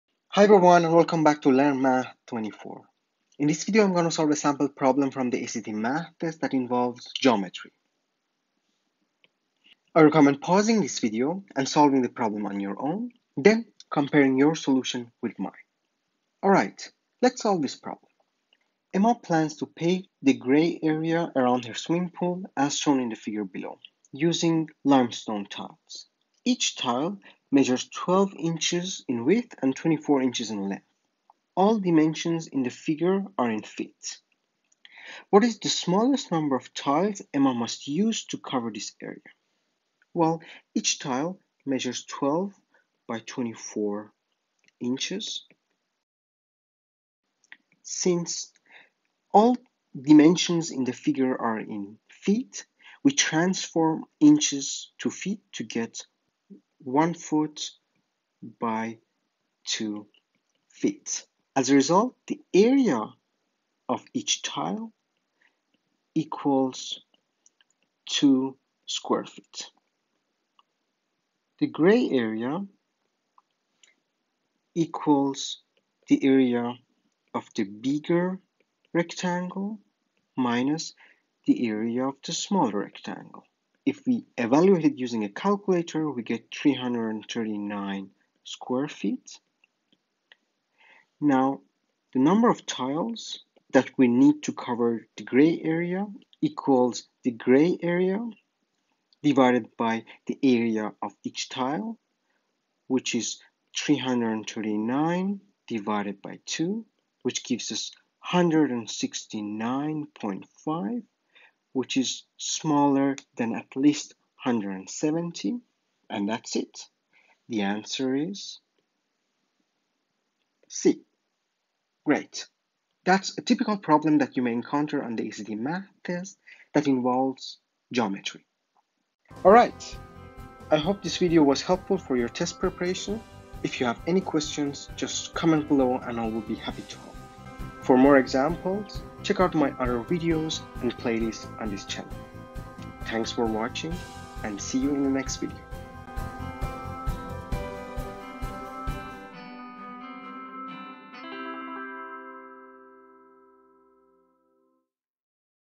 ACT Math - Geometry 📐 - Word Problem: Covering an area with tiles
Welcome back to @LearnMath24! In this video you will learn how to solve a sample word problem from the ACT Math Test that involves geometry 📐.

This video is a valuable resource for ACT Math Test preparation. My goal is to provide a concise and accessible problem walkthrough to help you excel in the ACT Math Test.